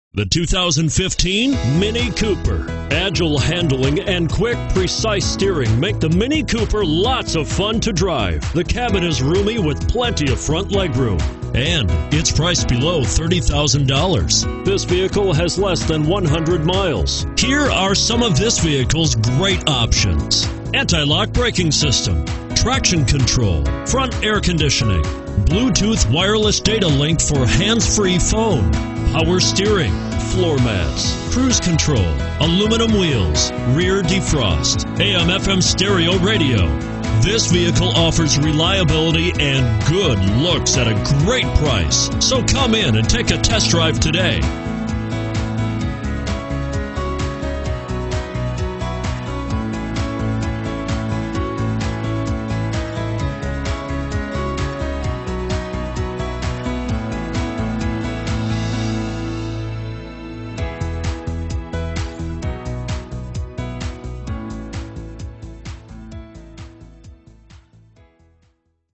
2015 MINI Cooper Hardtop Kennesaw GA M7268
2015 MINI Cooper Hardtop S - $26102

You'll love this 2015 MINI Cooper Hardtop. This is a vehicle you'll want to take home. With 12 miles, it features Automatic transmission and an exterior color of Pepper White. Call and get in touch with MINI of Kennesaw directly, and be the first to open the vehicle door today!